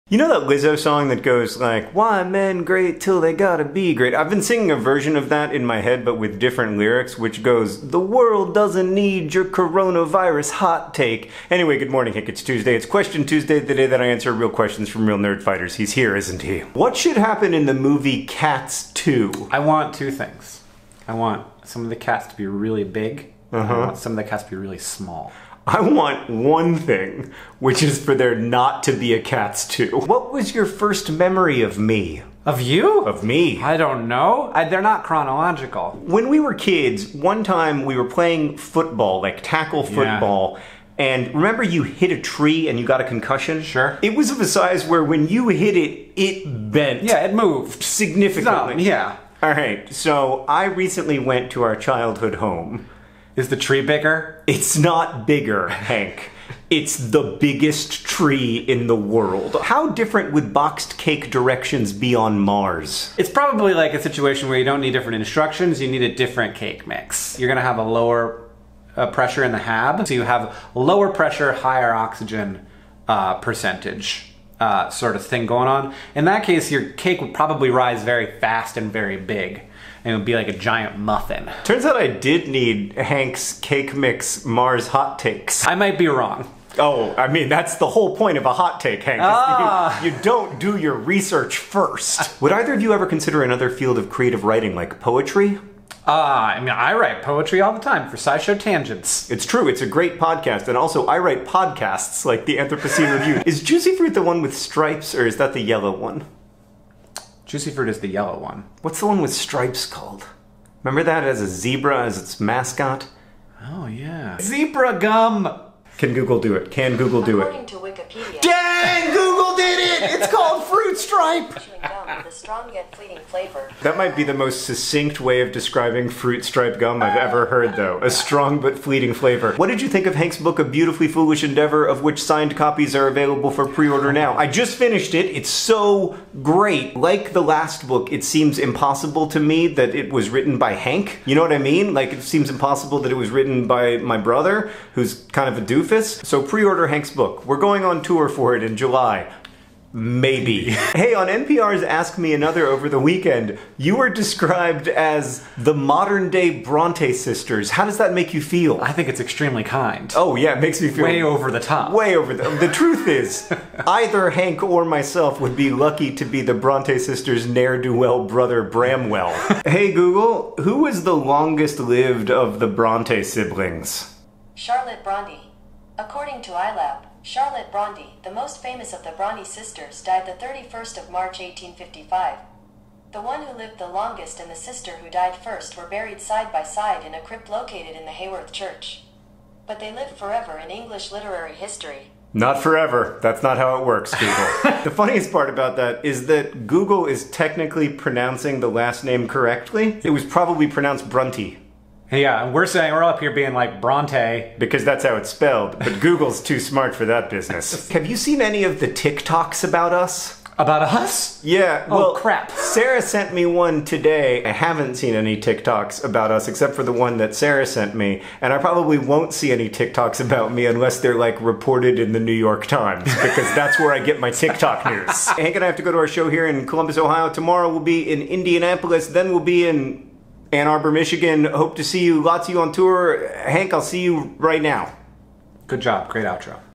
A Strong but Fleeting Flavor: A Reunion Video
In which John and Hank reunite to discuss Juicy Fruit, childhood trees, and the Bronte sisters' ne'er-do-well brother.